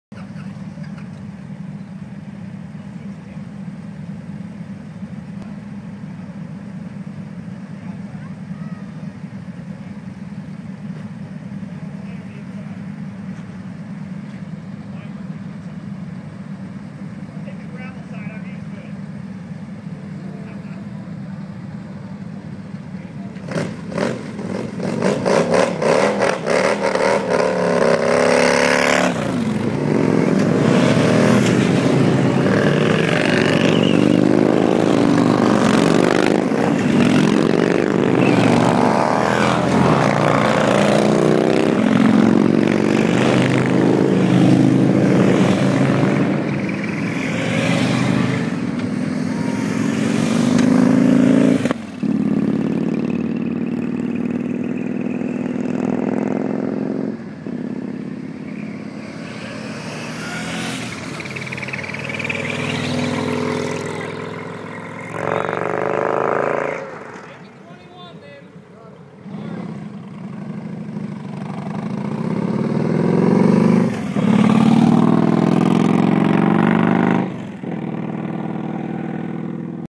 The First Wave - 21 Bikes leaving Starkey Station headed for the 5th Annual Bronze Bike Rally at Joseph, OR.